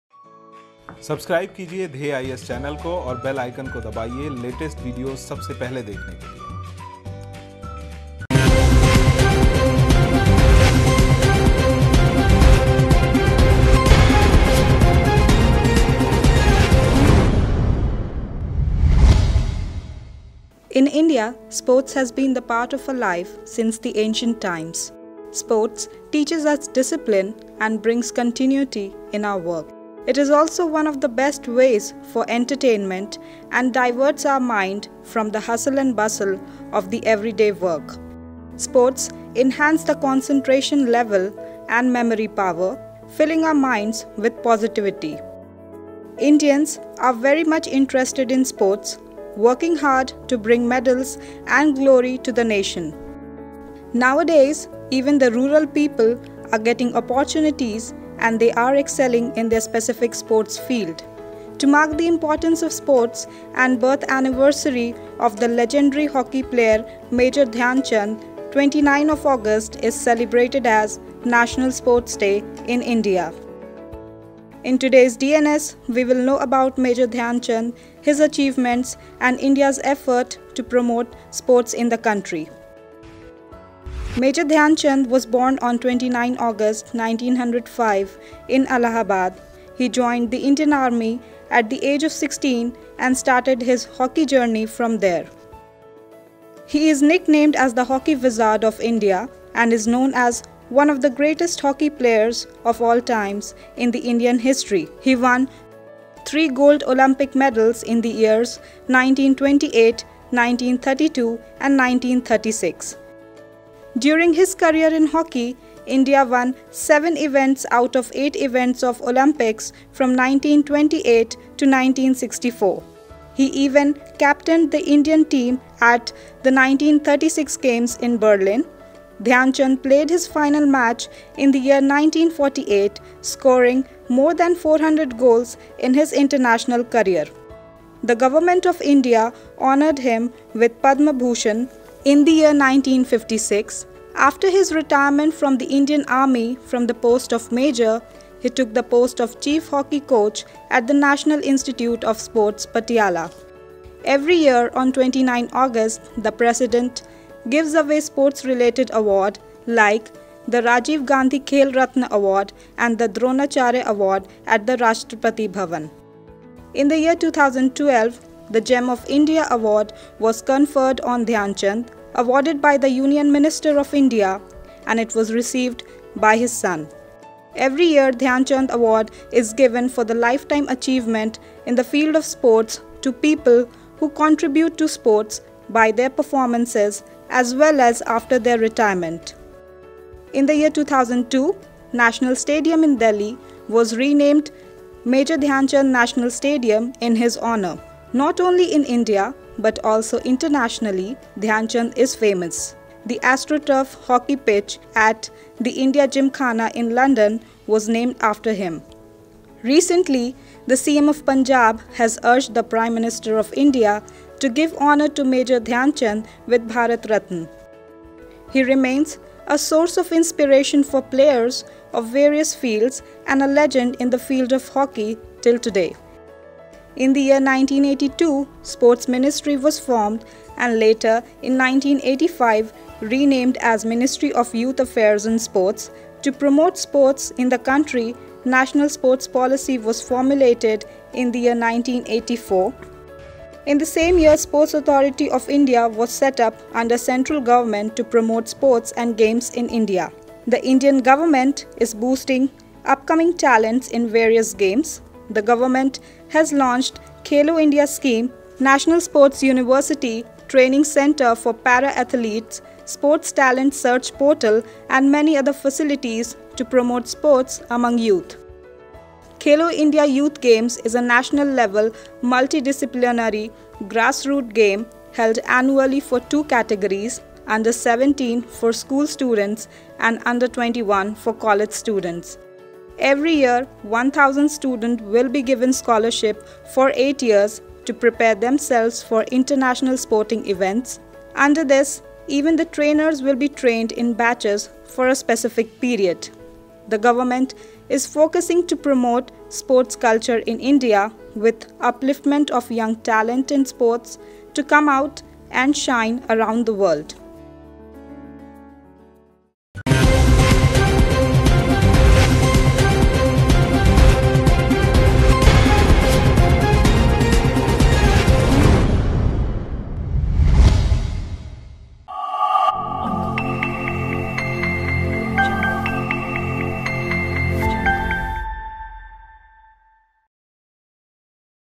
DNS: NATIONAL SPORTS DAY
In India sports has been the part of our life since the ancient times.
Sport teaches us discipline and brings continuity in our work. It is also one of the best ways for entertainment and diverts our mind from the hustle & bustle of the everyday work. Sports enhance the
concentration level and memory power, filling our minds with
positivity.  Indians are very much interested in sports, working hard to bring medals and glory to the nation. Now a day’s, even the rural
people are getting opportunities and they are excelling in their specific sports field. To mark the importance of sports and birth anniversary of the legendary hockey player Major Dhyan CHAND; 29 of August is celebrated as National Sports DAY in India.

In today’s DNS we will know about Major Dhyan Chand his
achievements and India’s effort to promote sports in the country.

VOICE AND REPORT: SHIBA
GRAPHICS: PANKAJ JAIN
EDITOR: SONAL MISHRA